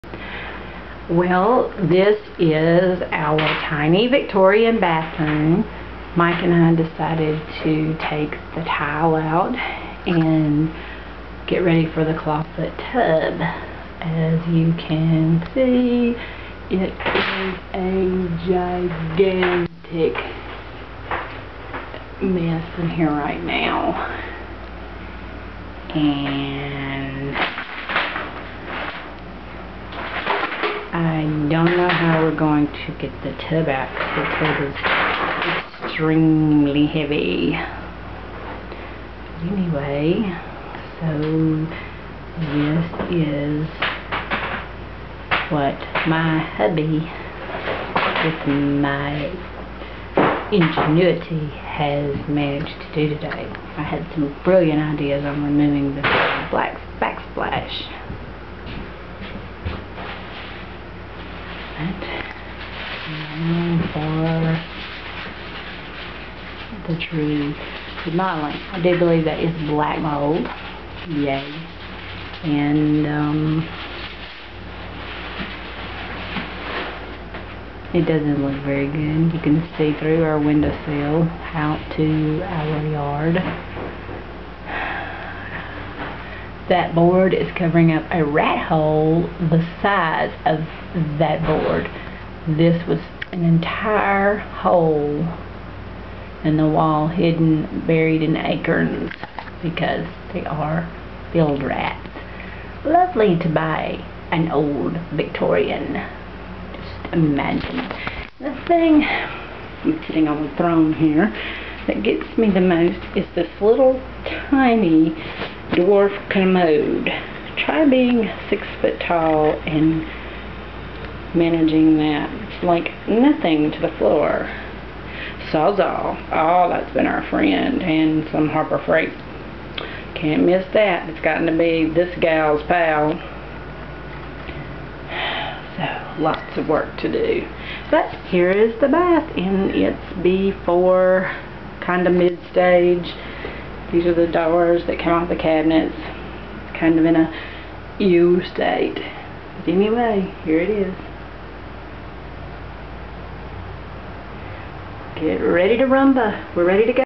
c ...begining bath restoration
Restoring a 112 year old house. Removing varmints. lol